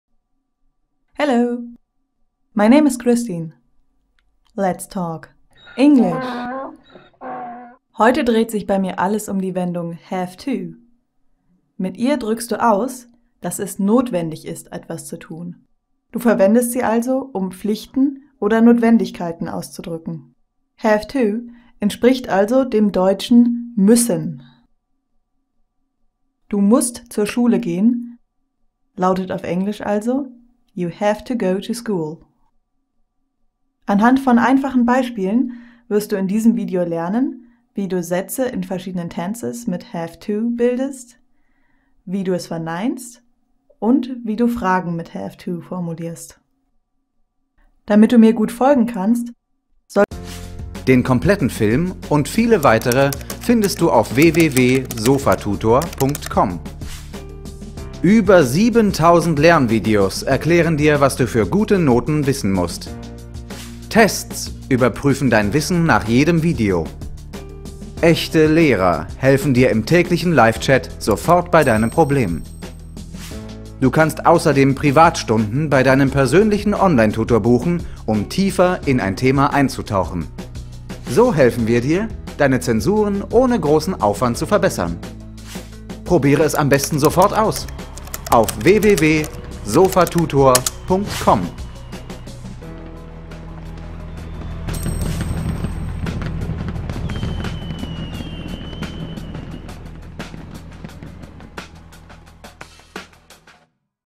Englisch: have to drückt Notwendigkeiten und Pflichten aus | Grammatik
Im Englischen werden Notwendigkeiten und Pflichten mit der Redewendung have to/has to ausgedrückt. Doch wie genau wird diese Wendung eigentlich benutzt? Dieses Video beschäftigt sich mit diesem Thema und wird dir diese Frage beantworten können. Du wirst diesen Ausdruck mit Hilfe von einigen Beispielen genau kennenlernen. Dir wird in diesem Video gezeigt, wie have to/has to im Englischen in die verschiedenen Zeitformen, wie z.B. Simple Present oder Simple Past, übertragen wird. Du wirst auch lernen, wie du die Wendung auf Fragen und Verneinungen (negativen Sätzen) anwenden kannst. Es gibt in diesem Film Übungen, mit denen du testen kannst, ob du alles verstanden hast. Viel Spaß!

Dieses Video zum Thema Satzbau und Satzgefüge gehört zu: Englisch | Grammatik | Satzbau und Satzgefüge